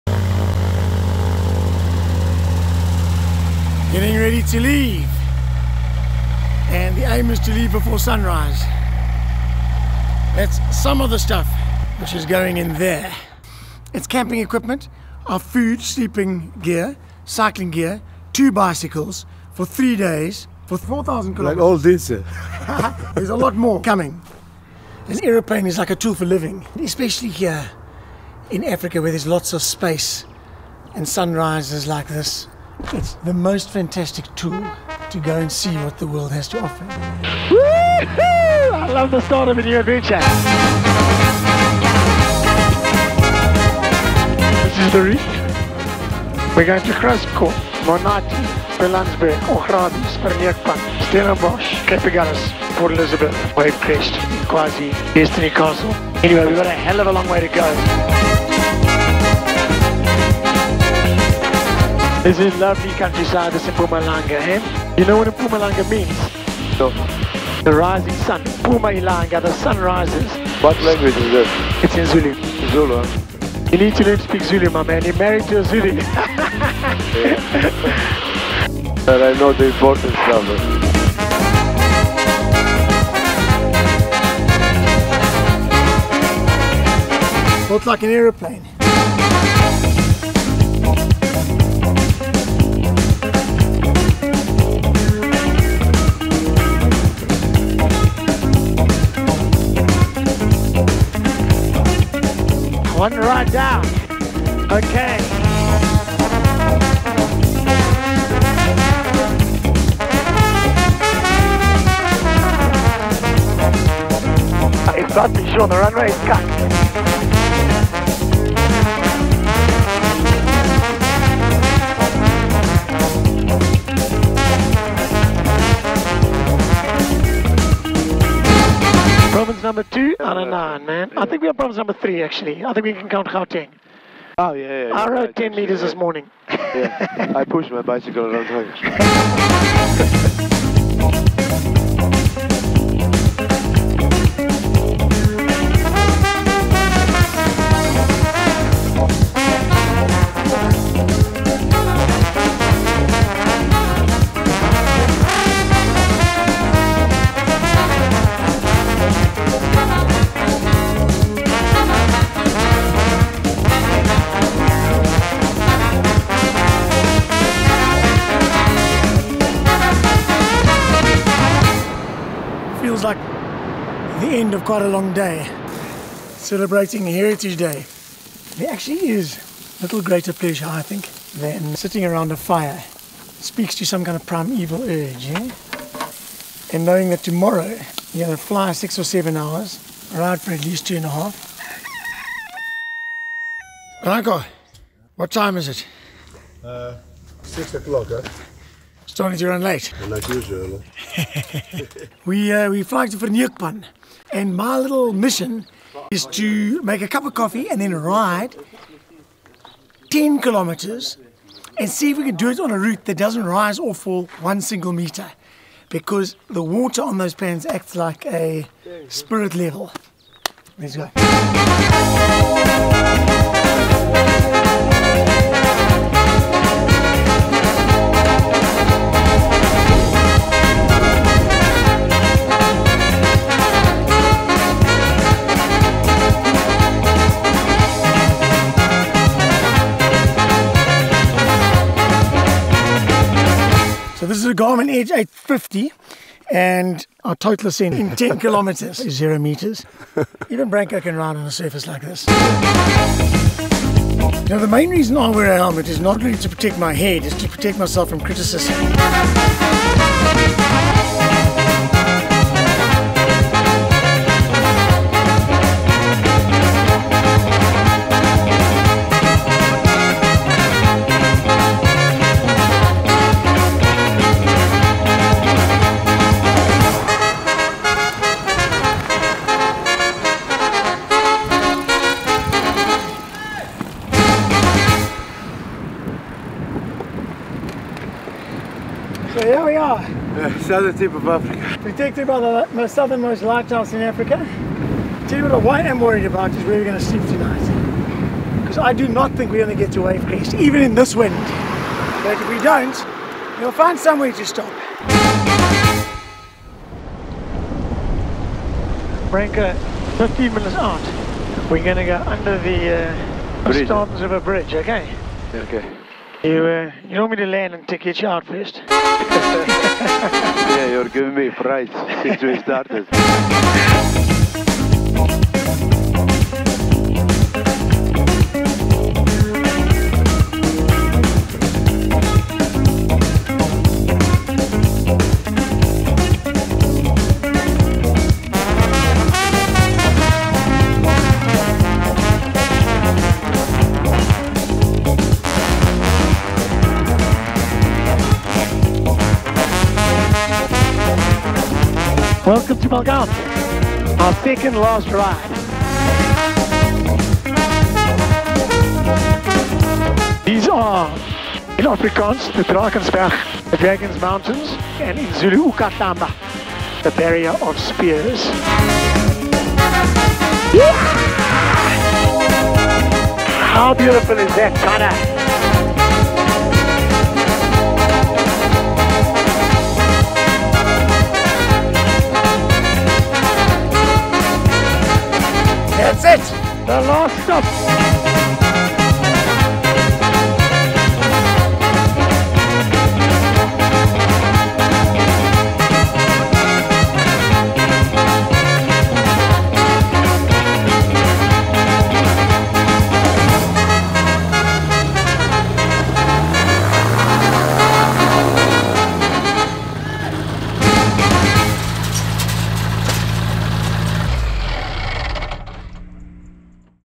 Around South Africa in 3 Days
James Pitman and Branko Brkljac of Sling Aircraft attempt to fly around South Africa and ride their prototype Sling Tagati bicycles in each of the country's 9 provinces over the 3 day Heritage long weekend. James and Branko are in the factory Sling TSI and filmmaker Lloyd Ross is in the prototype Sling High Wing, piloted by Sean Russell.
Copyright Shifty Media